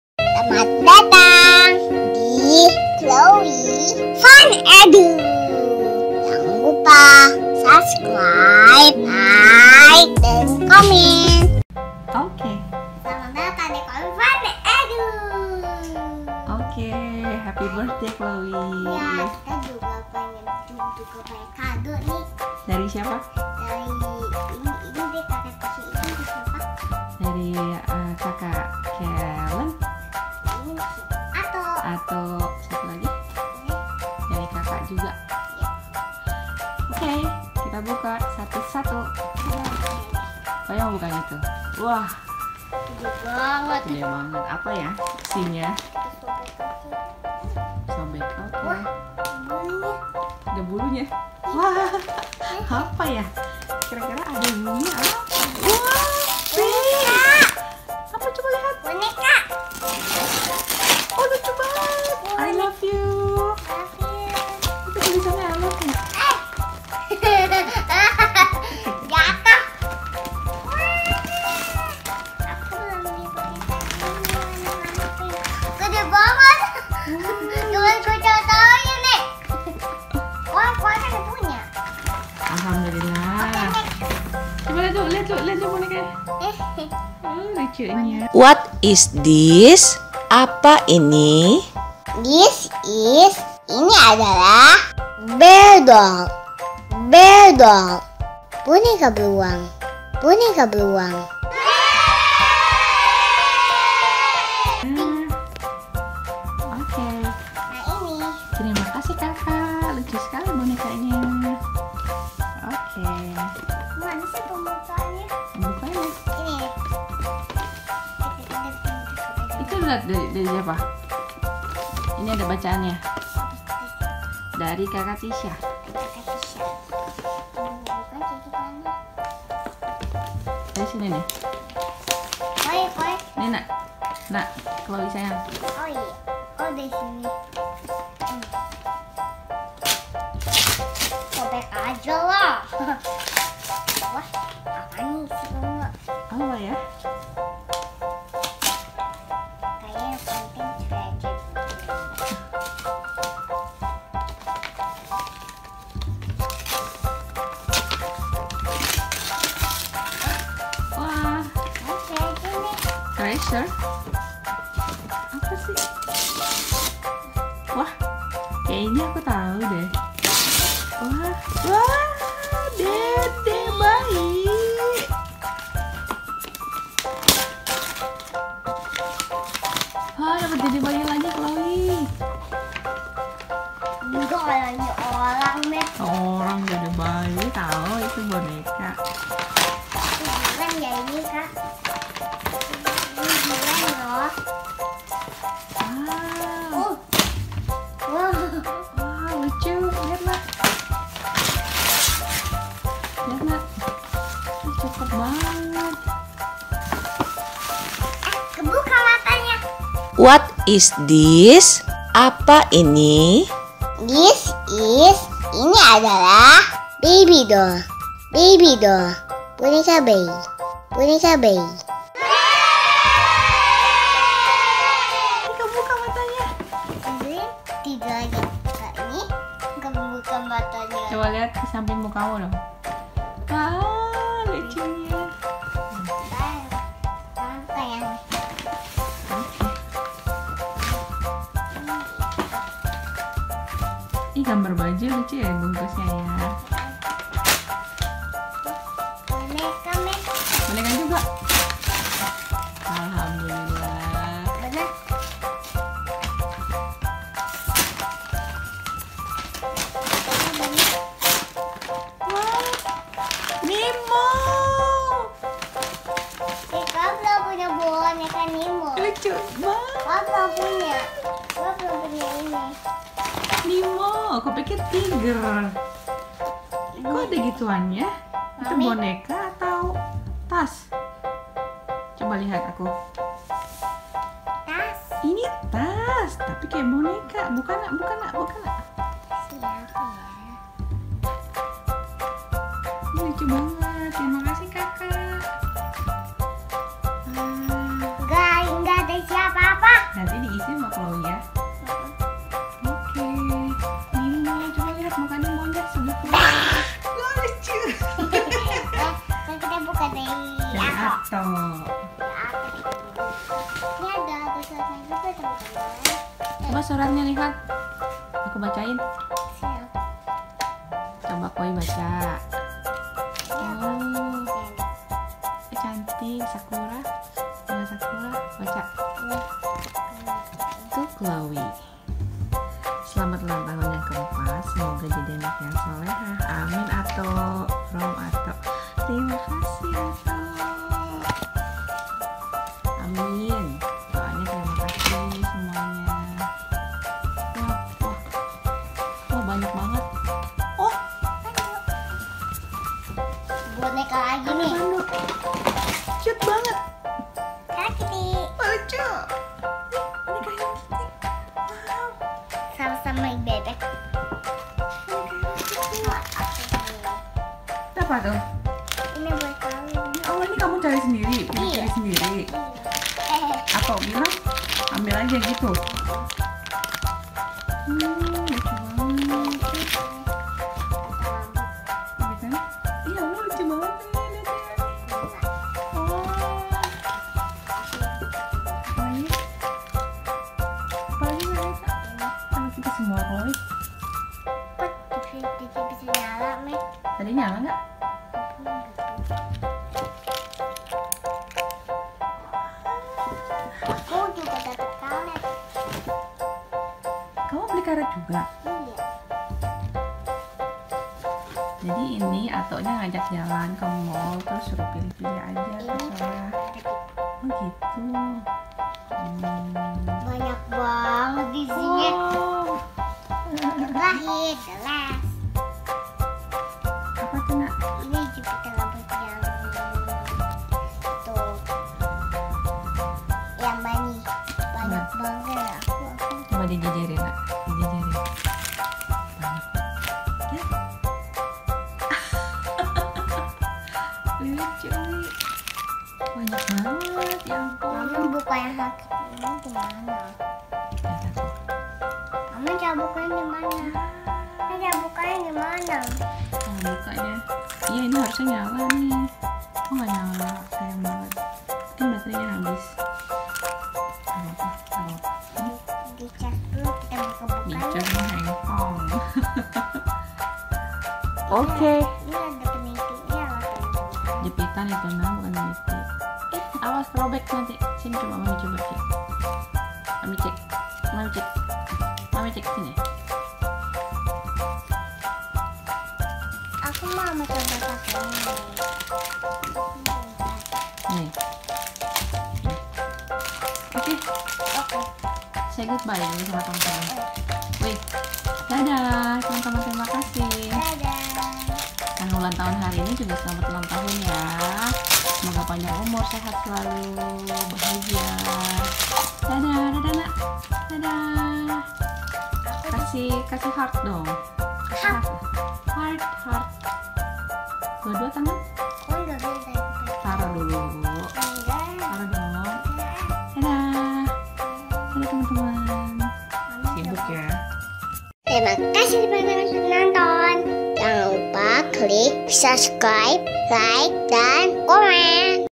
Buka Kado Ulang Tahun Chloe Yang Ke-4 Tanggal 5 Juli 2018
Alhamdulillah Chloe ulang tahun yang ke-4 dan bakal buka kado-kado ulang tahunnya. Kira-kira isi kado-kadonya apa aja ya? Yuk kita tonton aja teman-teman.